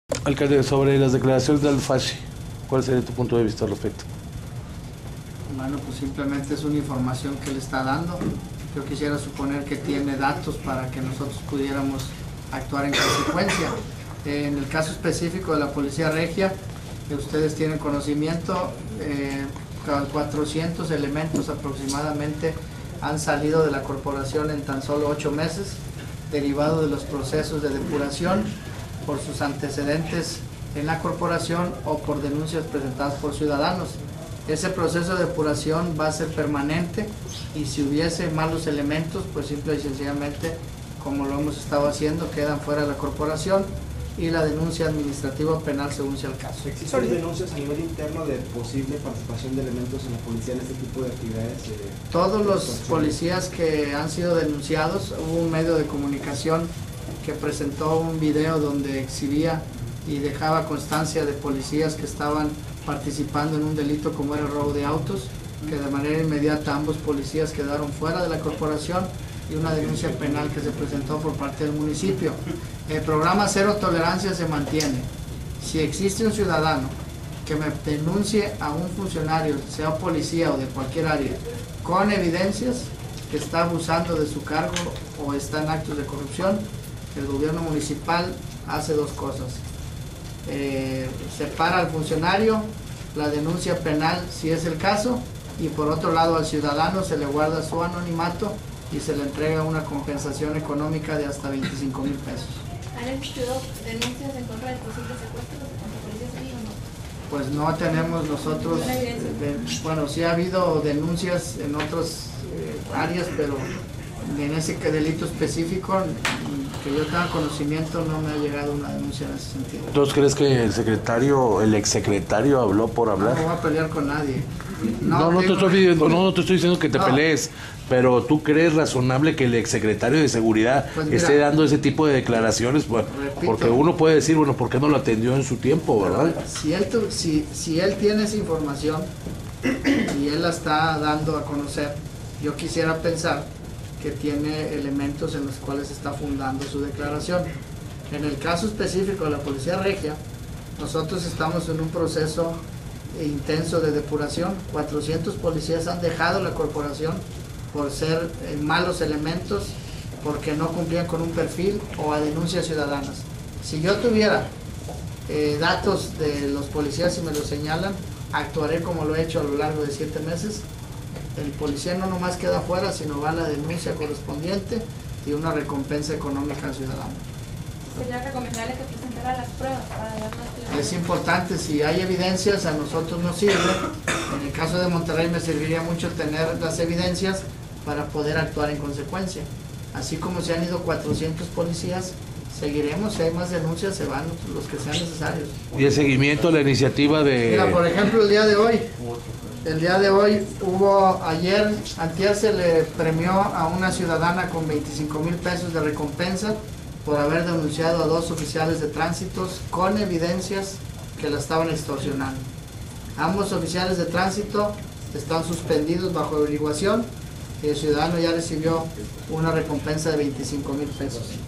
Fasci deberá sustentar sus acusaciones: Larrazabal
Antes las aseveraciones hechas por el ex secretario de Seguridad Pública, Aldo Fasci, en el sentido de que elementos de las corporaciones policíacas tienen complicidad con el crimen organizado; el alcalde Fernando Larrazabal señaló que si el ex funcionario cuenta con pruebas debería de presentarlas para iniciar con ello una investigación.